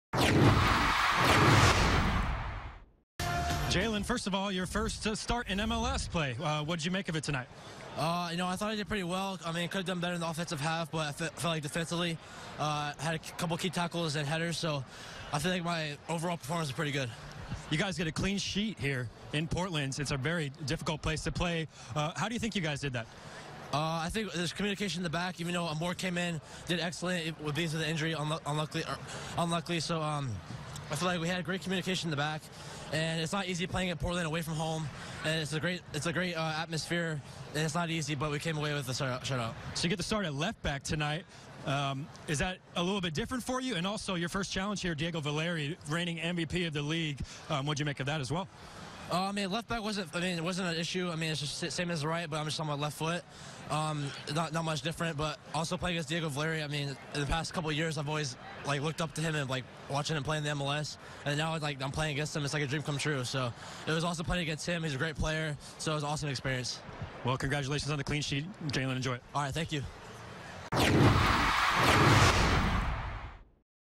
Jaylin Lindsey: "It's like a dream come true" playing against Diego Valeri
Jaylin Lindsey after his first MLS start: "I thought I did pretty well. I could've done better in the offensive half, but I felt like defensively I had a couple key tackles and headers."